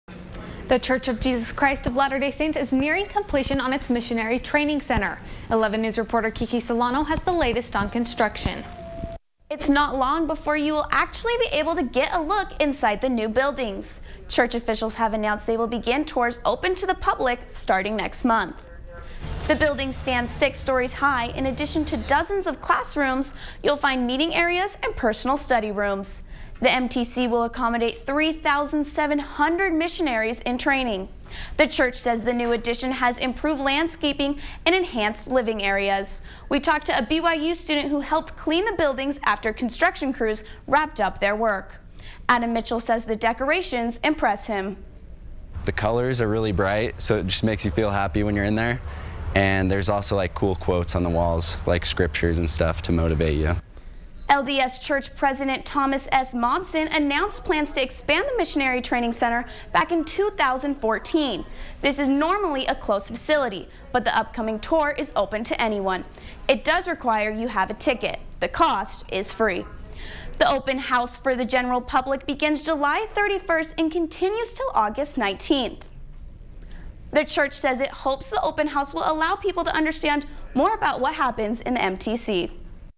Public tours announced for Missionary Training Center
The Church of Jesus Christ of Latter-Day Saints has announced that guided tours of the Missionary Training Center next month following the construction of a new addition to the complex.